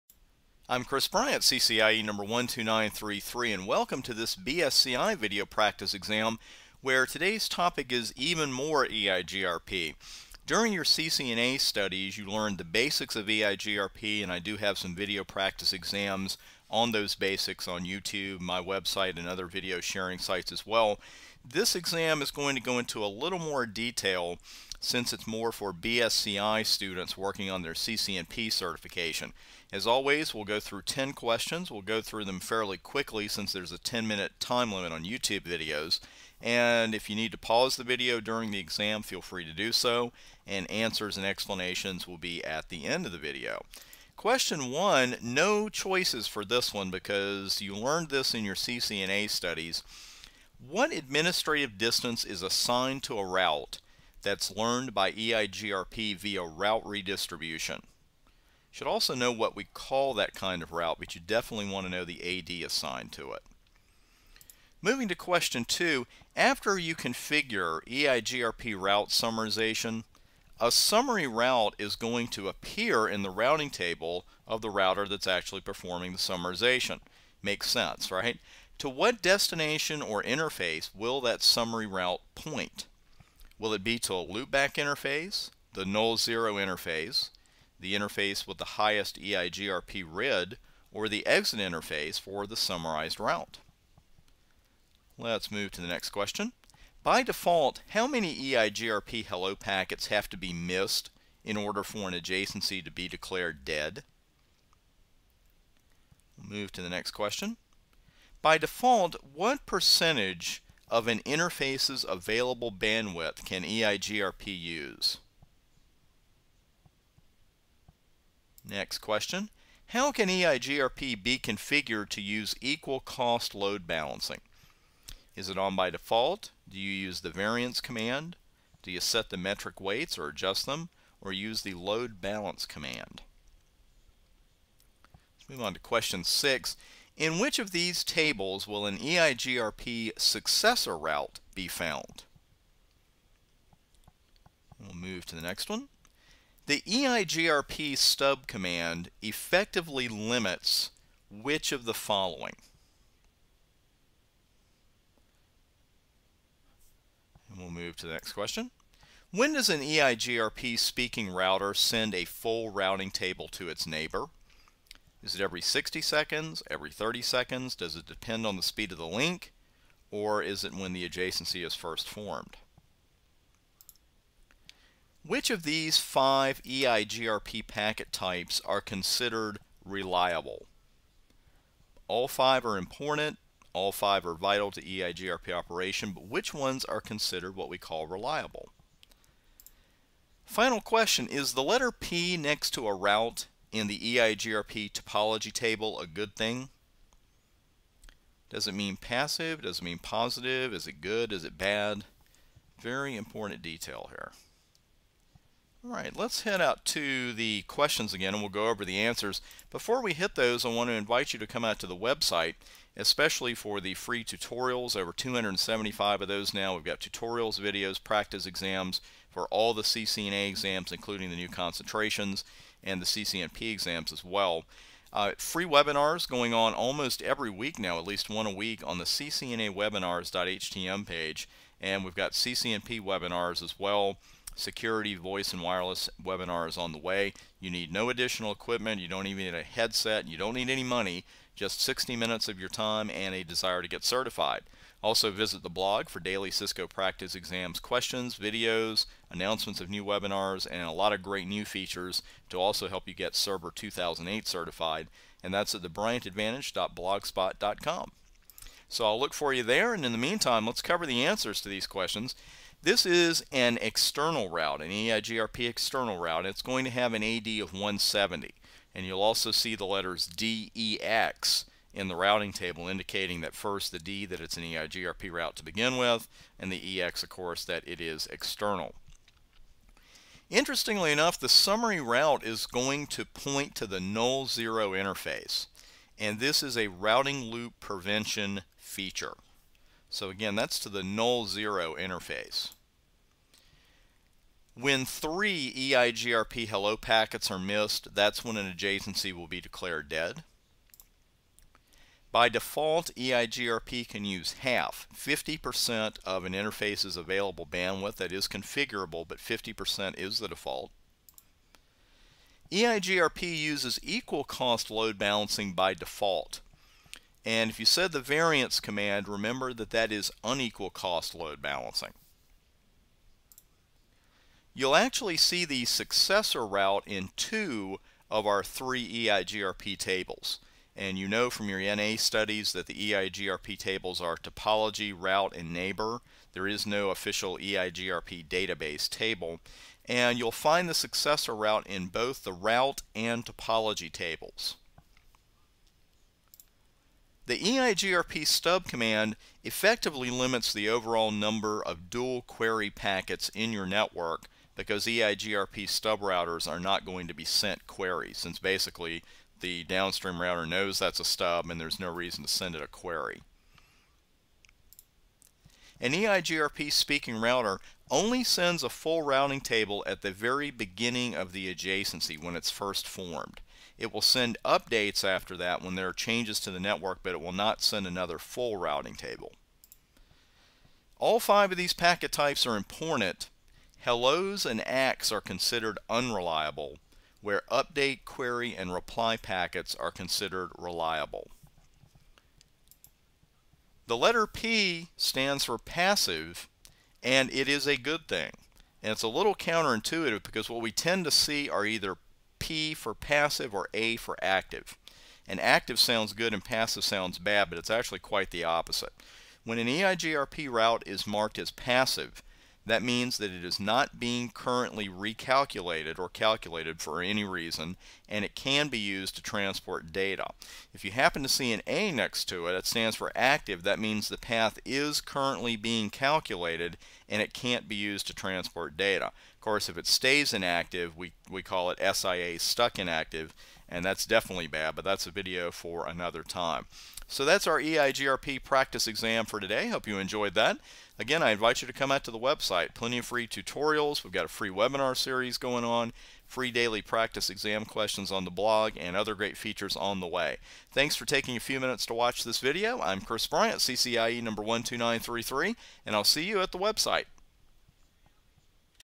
CCNP BSCI Video Practice Exam : EIGRP
To help you prepare for BSCI exam success, this CCNP Video Practice Exam will present you with 10 questions on EIGRP.  Answers and full explanations are given at the end of the video.

to register for free CCNA and CCNP tutorials taught by yours truly!

Chris Bryant
CCIE #12933